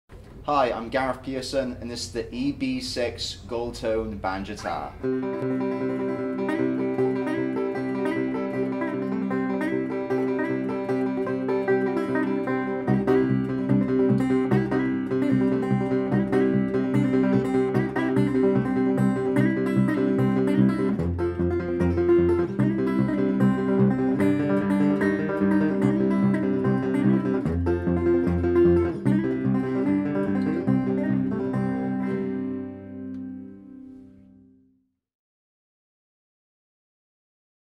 Electric Banjo Guitar! Gold Tone EB-6!
The EB-6 Banjitar's tone is generated by a custom Remo pre-tuned eight-inch banjo head that never needs re-tensioning, providing enhanced tuning stability. The contour-carved mahogany body and Canadian maple neck with rosewood fretboard make for a nicely balanced banjo guitar that won't be a burden after long sets. The stacked humbucking pickup is highly feedback-resistant and faithfully captures the plunky banjo tone and fast decay of this versatile instrument. The heavy Terminator tailpiece accept ball-end or loop-end strings. Available as left-handed model.